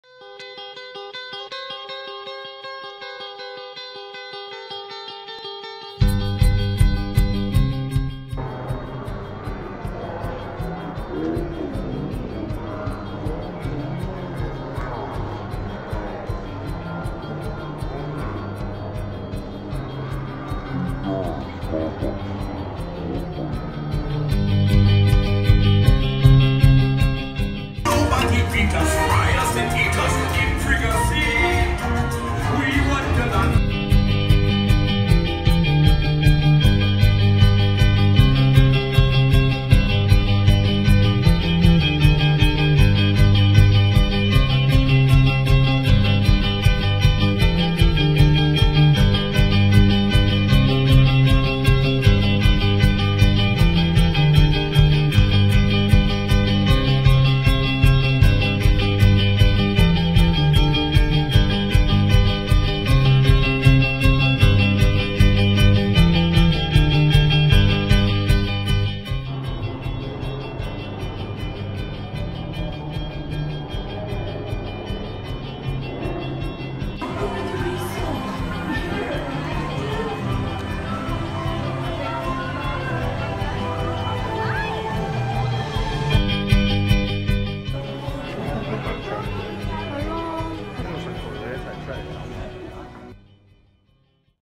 Disney California Adventure| An Afternoon at LA Disney Adventure
We went there last year and got some time to make this little video during out stay home days. Hope can bring y’all some joy.